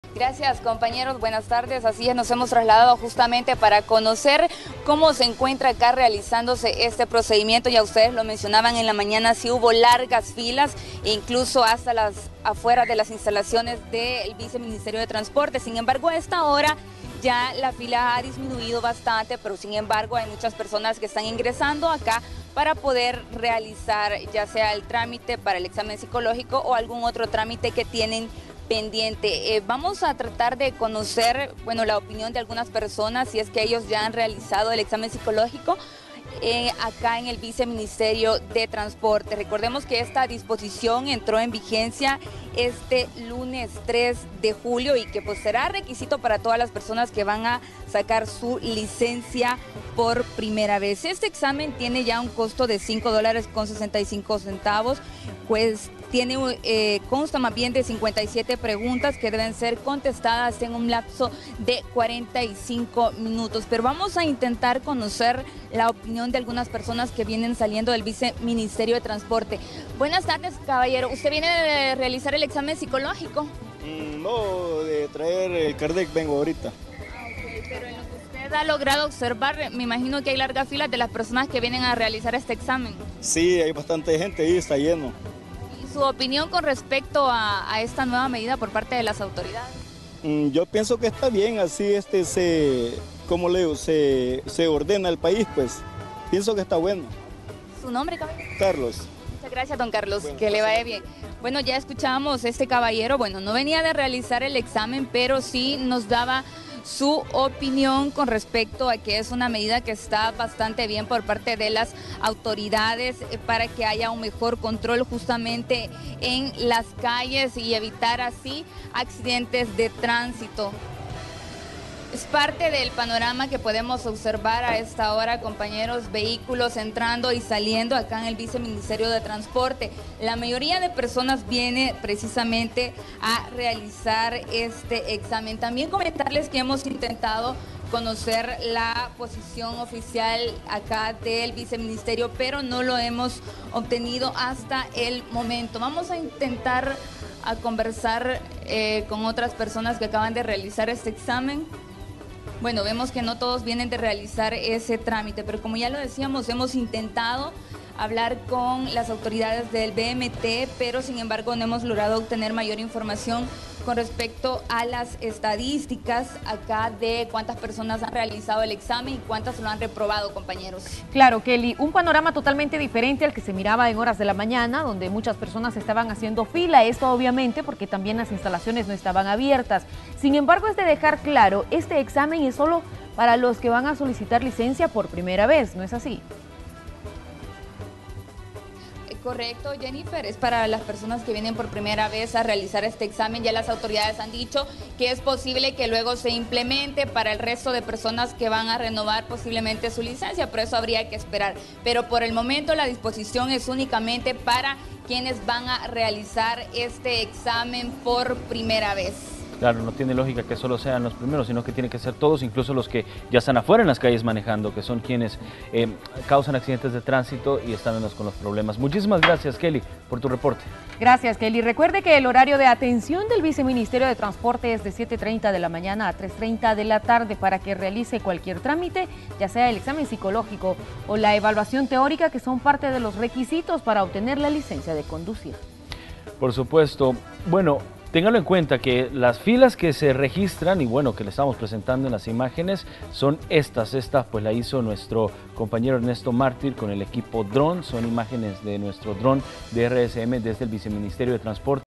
Decenas de personas hacen largas filas en el VMT
Luego de la implementación del examen psicológico, que cuesta 5 dólares con 65 centavos, y que tiene 57 preguntas que deben ser contestadas en 45 minutos, las personas han comenzado a realizar el trámite para conseguir por primera su licencia de conducir. ¿Qué opina la gente al respecto?